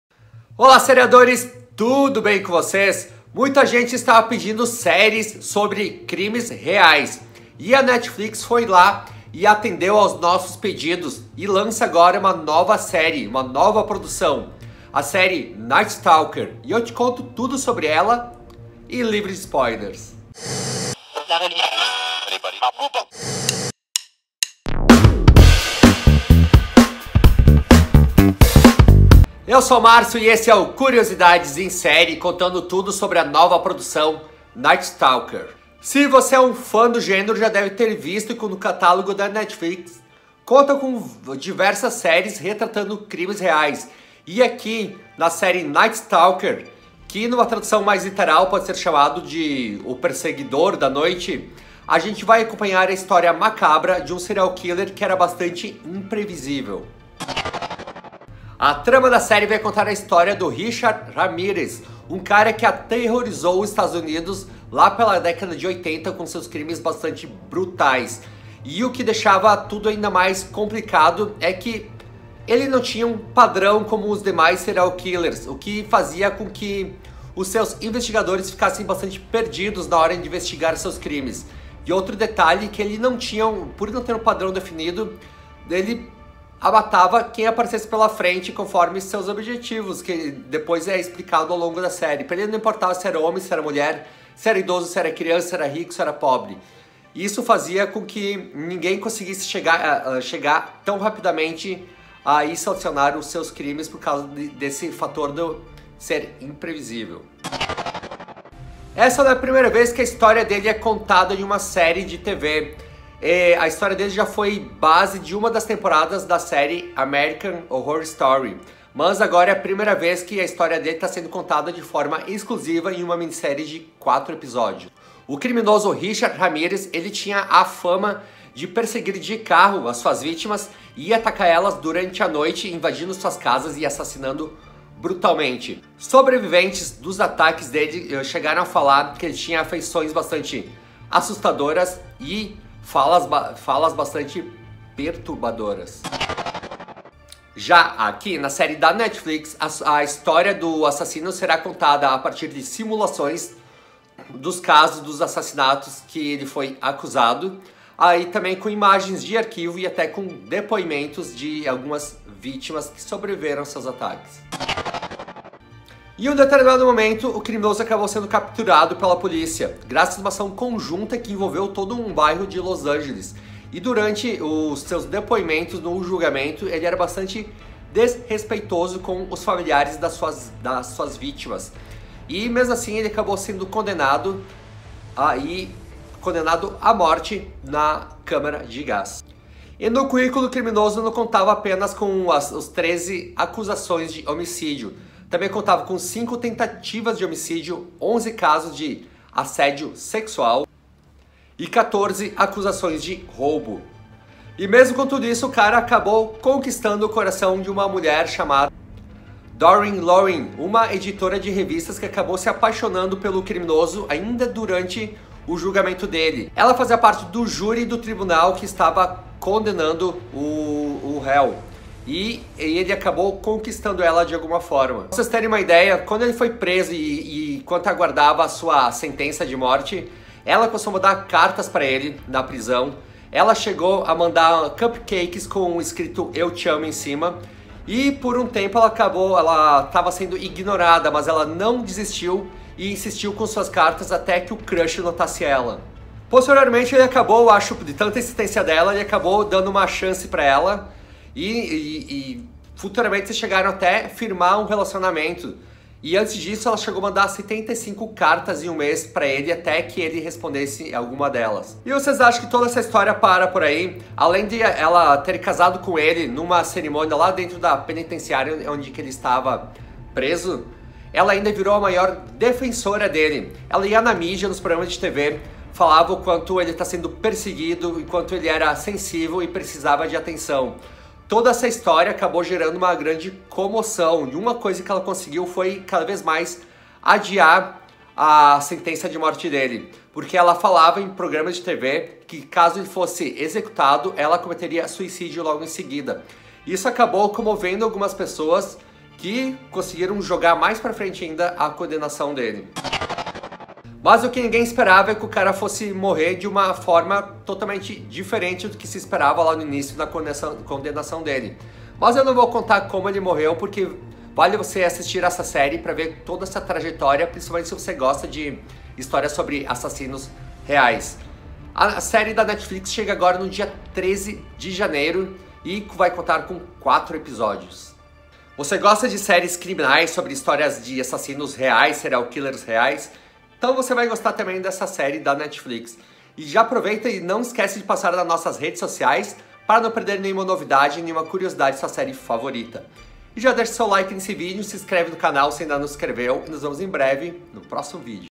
SAIBA MAIS SOBRE A SÉRIE NIGHT STALKER
A série documental fala sobre a vida e os crimes reais cometidos pelo serial killer Richard Ramirez. Neste vídeo conto tudo o que você precisa saber sobre a série.

▶ Tem alguma opinião sobre o vídeo de hoje? Ou uma sugestão legal de tema para o próximo? Deixe nos comentários!

Roteiro e edição: Márcio de Oliveira